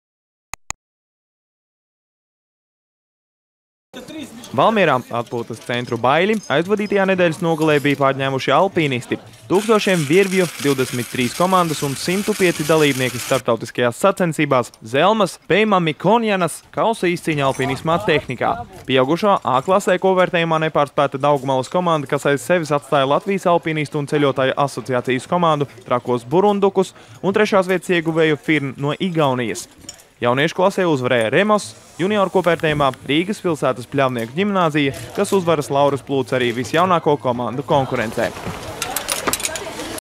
Zelmas Bej Ma Mikonjanas kausa izcīņa aplpīnisma tehnikā
Valmierā, atpūtas centru Baiļi aizvadītajā nedēļas nogalē bija pārņēmuši alpīnisti! Tūkstošiem virvju, 23 komandas un 105 dalībnieki starptautiskajās sacensībās -- Zelmas Bej Ma Mikonjanas kausa izcīņa aplpīnisma tehnikā!